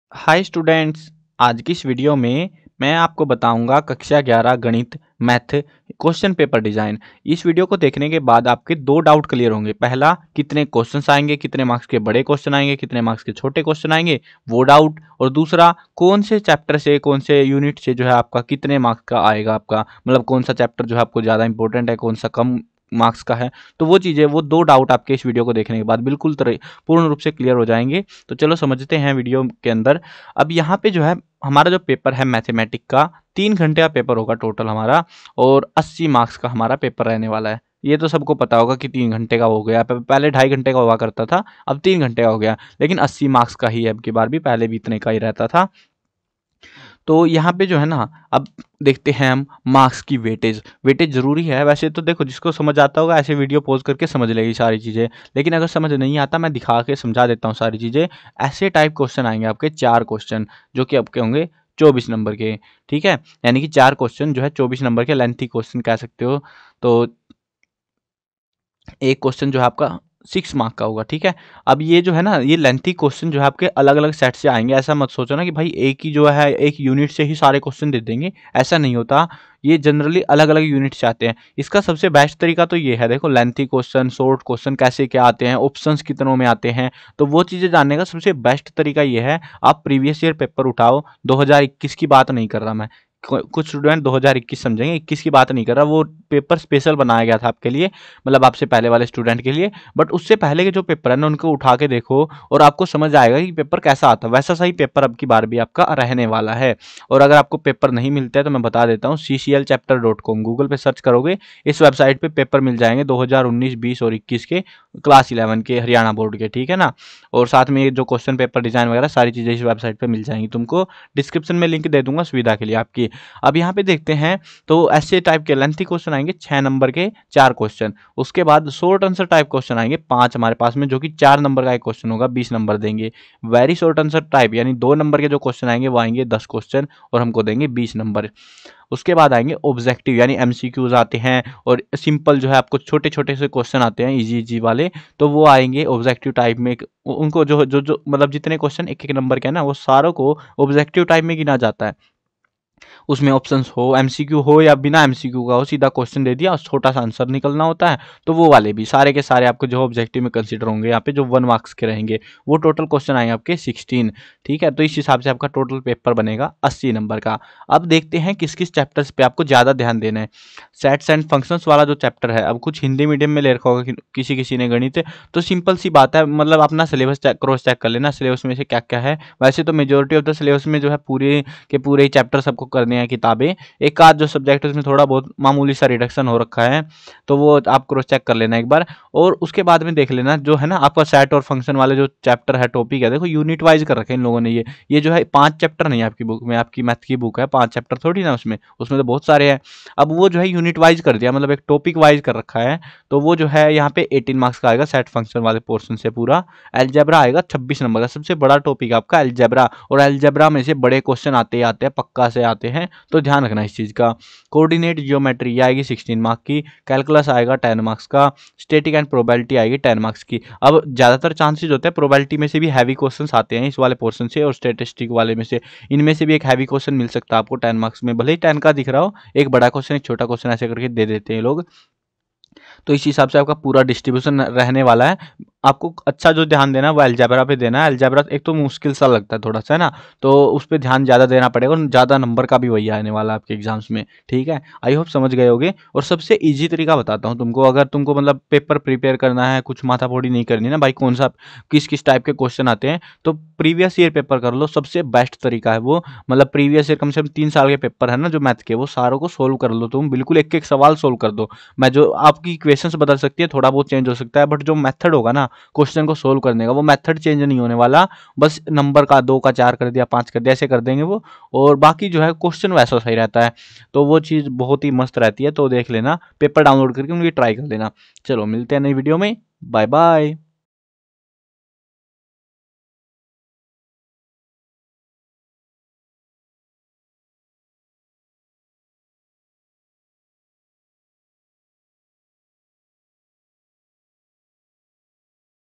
HBSE Class 11 Math Question Paper Design 2022-23 || March 2023 Question Paper Pattern class 11
Question Paper Design 2022-23 Class 11 Math for Haryana Board Students. Class 11 Math Question paper pattern 2023.